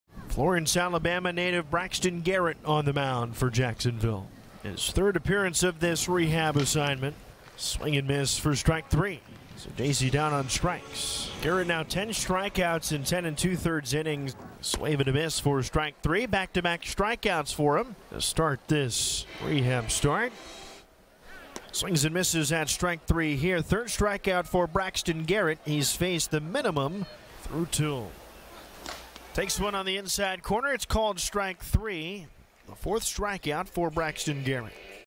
Braxton Garrett K's four | MiLB Highlights
4/25/24: Marlins southpaw Braxton Garrett fans four in a rehab start with Triple-A Jacksonville